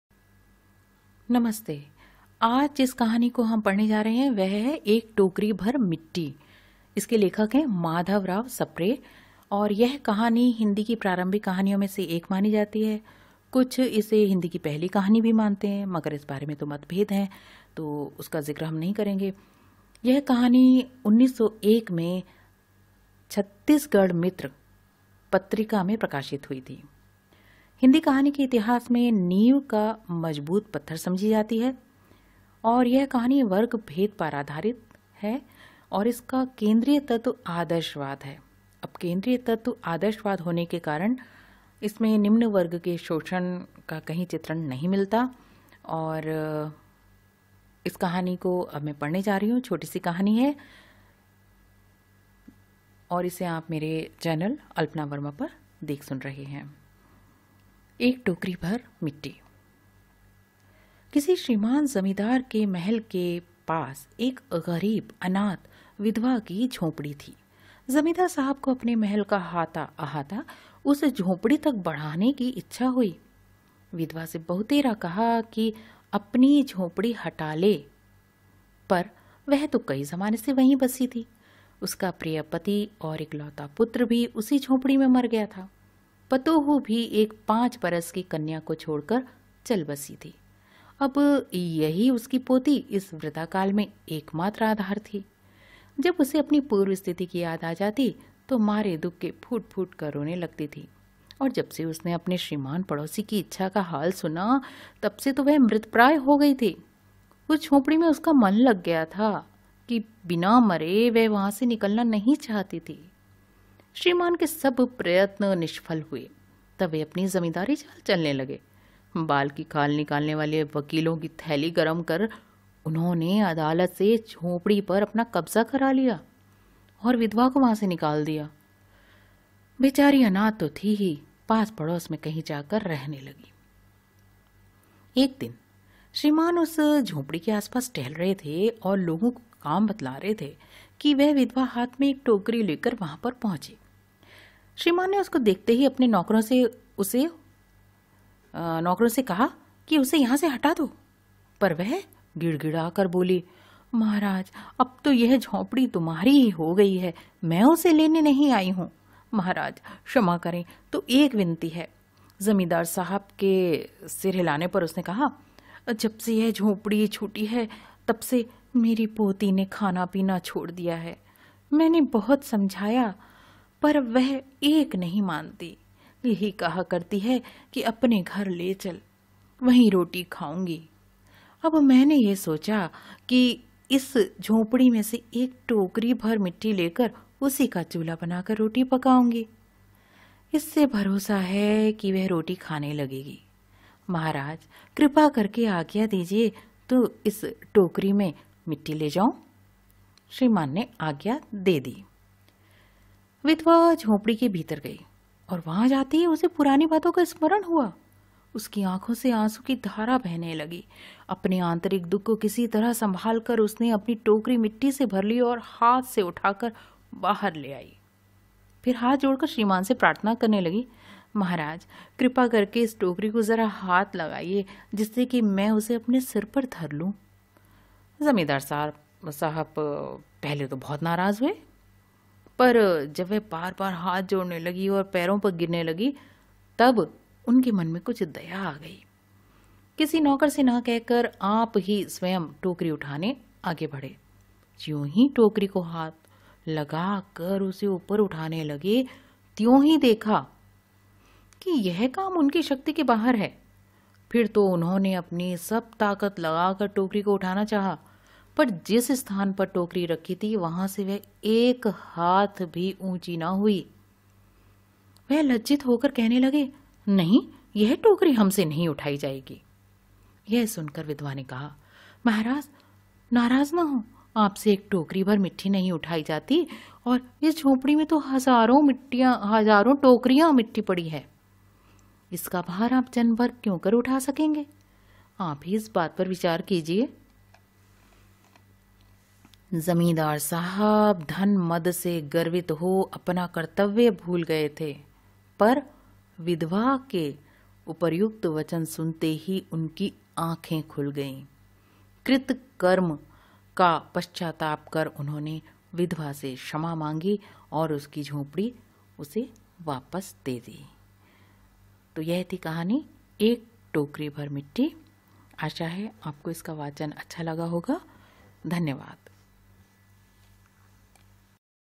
एक टोकरी -भर मिट्टी /माधवराव सप्रे /Ek Tokri bhar Mitti (hindi Story)
एक टोकरी -भर मिट्टी /माधवराव सप्रे
Ek Tokri bhar Mitti (hindi Story) वाचन स्वर : अल्पना वर्मा
UGC/NET -NTA   , Syllabus(Subject : Hindi) 2022
uploaded इकाई-VII   हिंदी कहानी
इस प्लेलिस्ट में उपलब्ध विडियो पाठों को रखा गया है :
Video prepared and presented by Alpana Verma